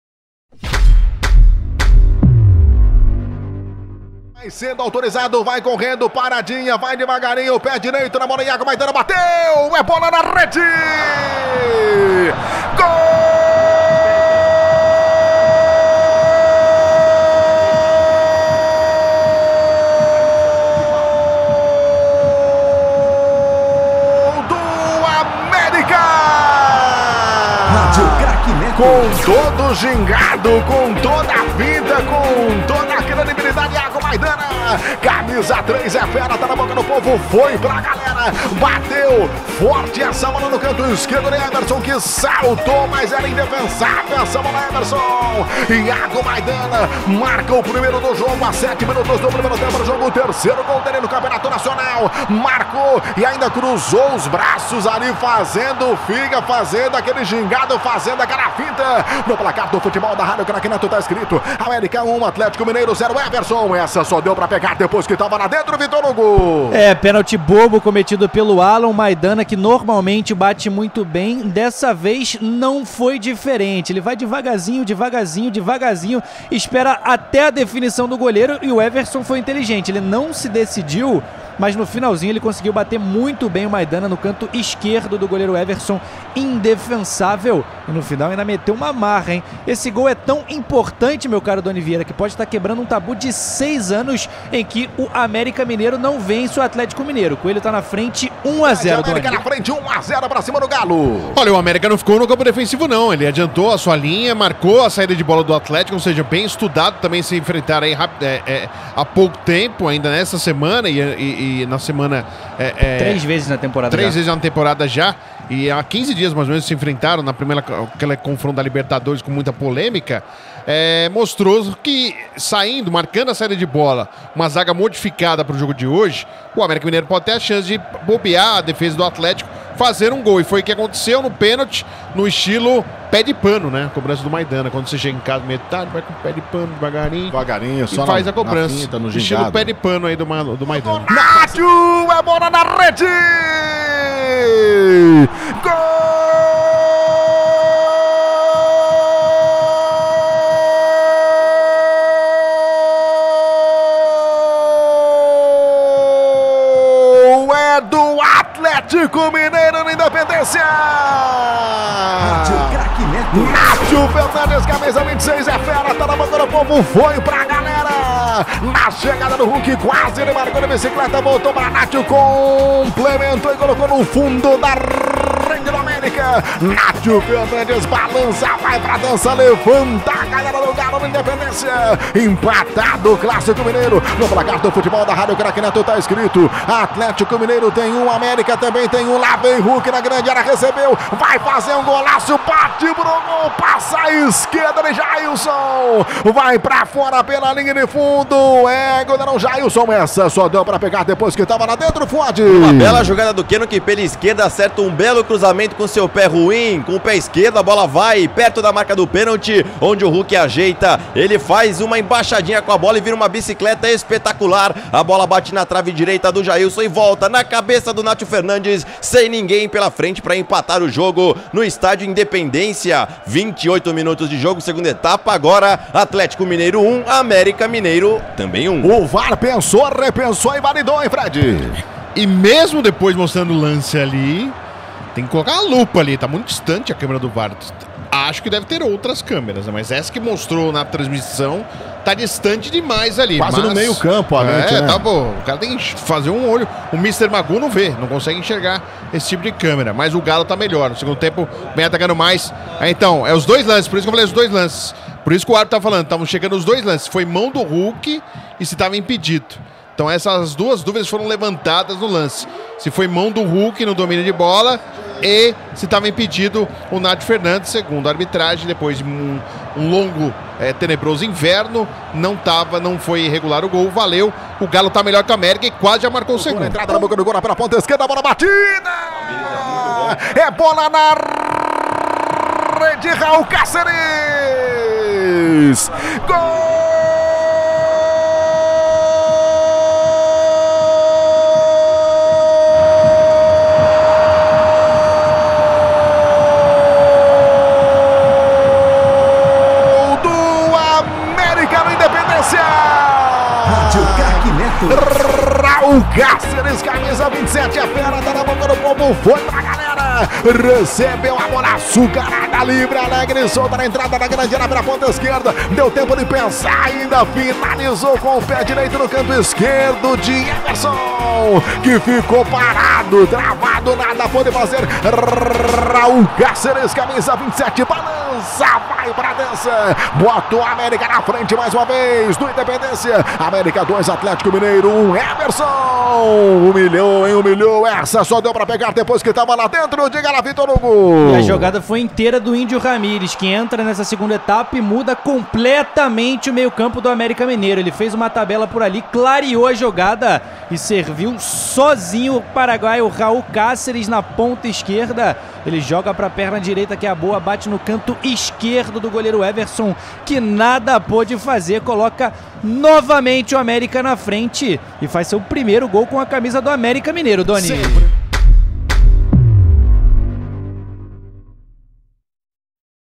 GOLS DE ATLÉTICO-MG X AMÉRICA-MG NA RÁDIO CRAQUE NETO
Narração dos gols de Atlético-MG x América-MG pelo Brasileirão